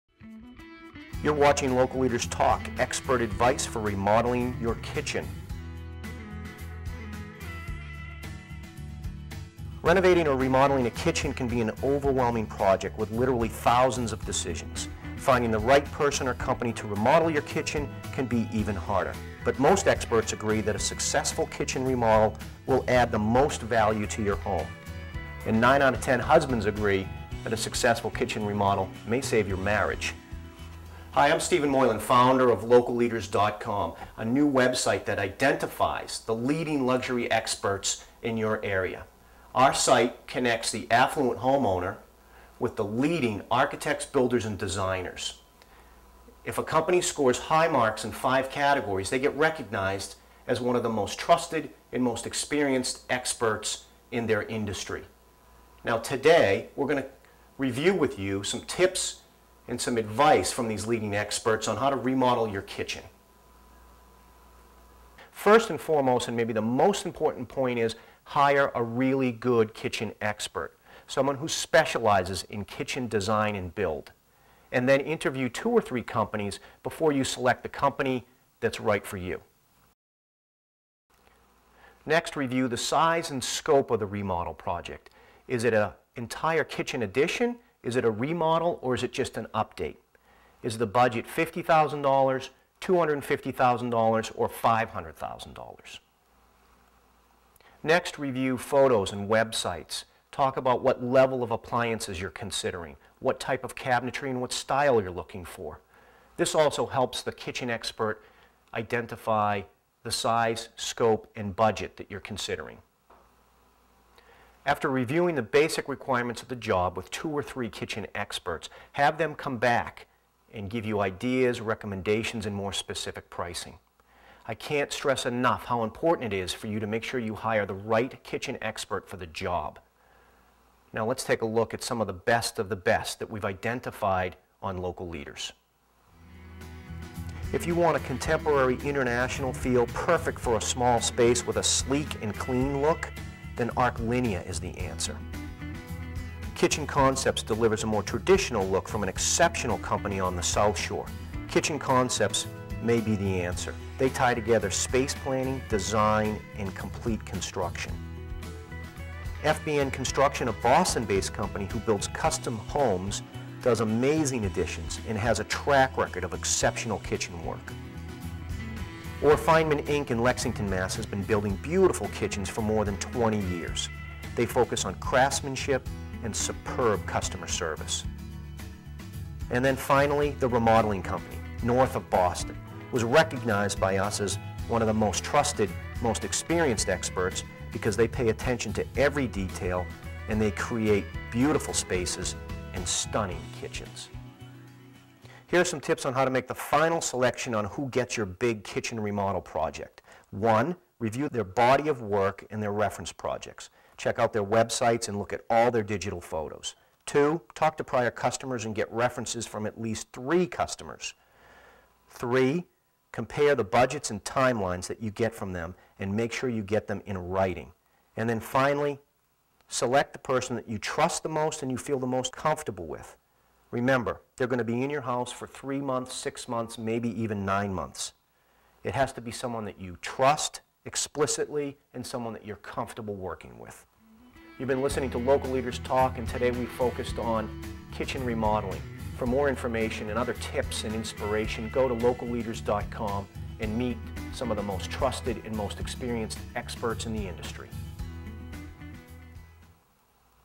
Kitchen Remodeling Tips (Part 1)
Tips for building, remodeling and renovating your home kitchen and suggestions of reputable companies to consider.